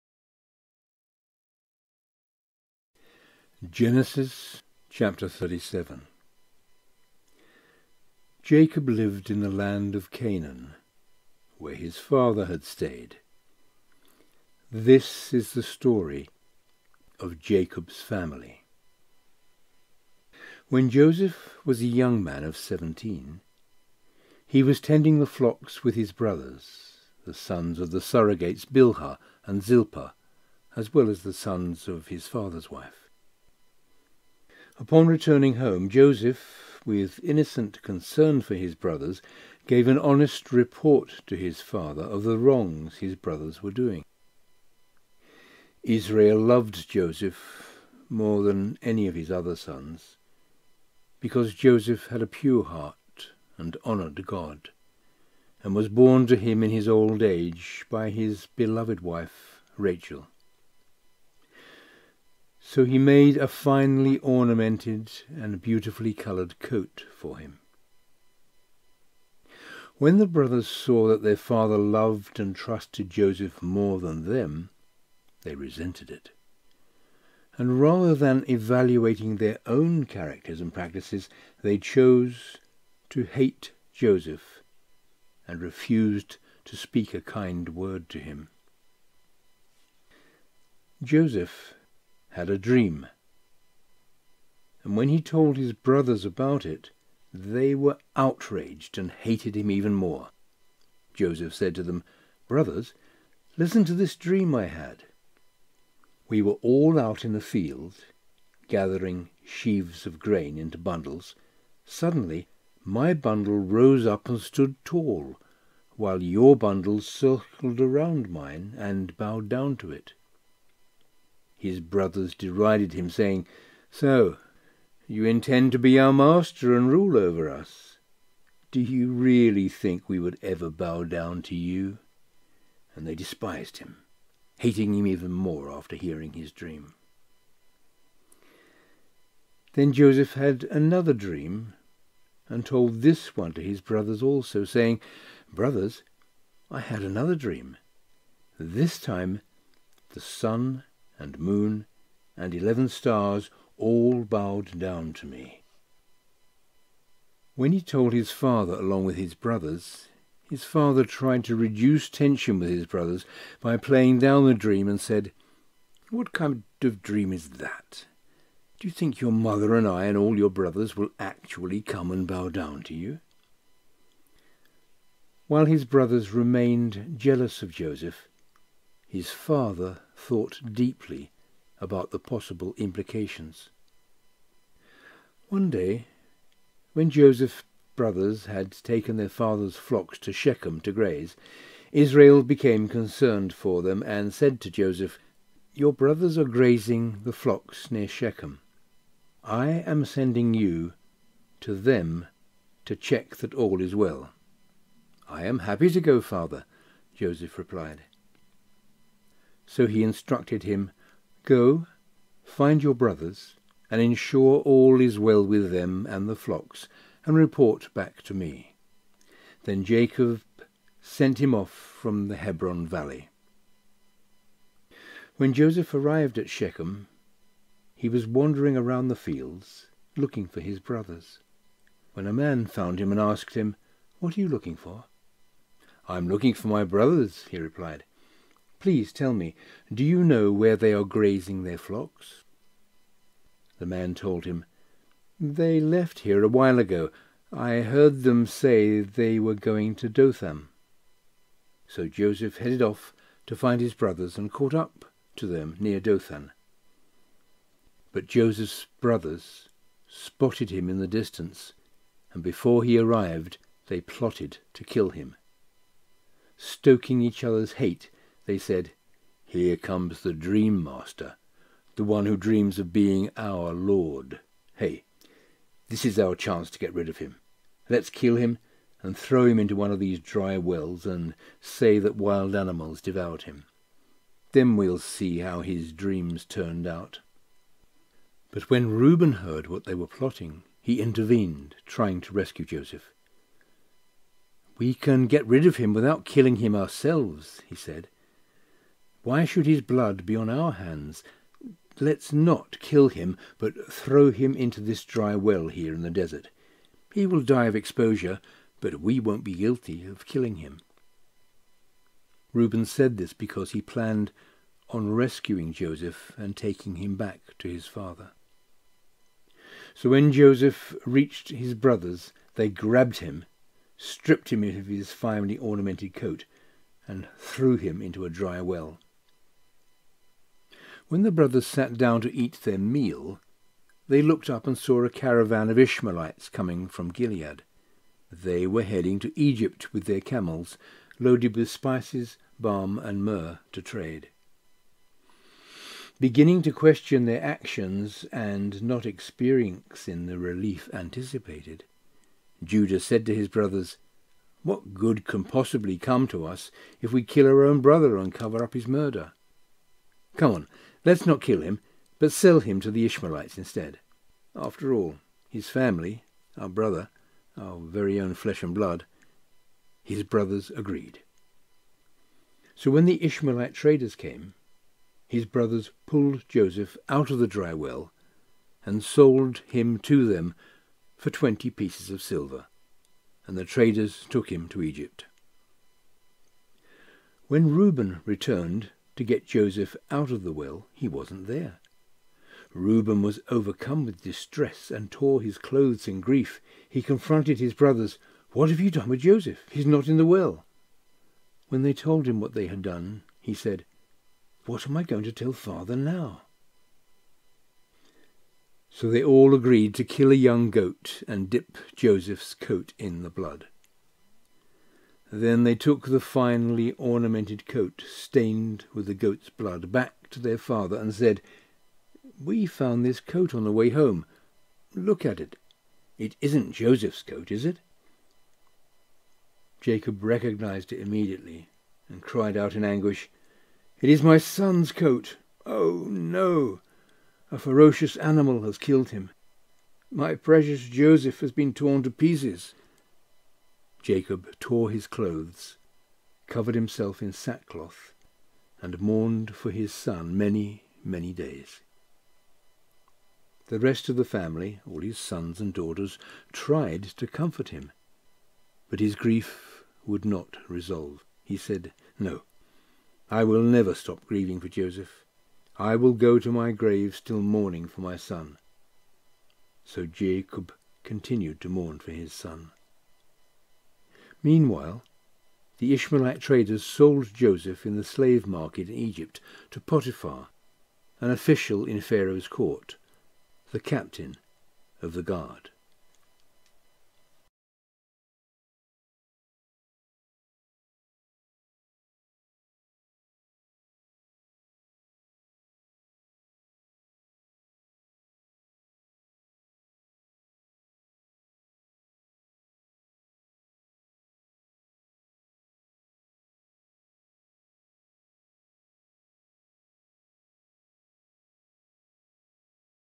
GENESIS Chapter 37 the Remedy - Joseph's blood-stained coat
Graphic background: daniel-posthuma-_Qin-dMNgFs-unsplash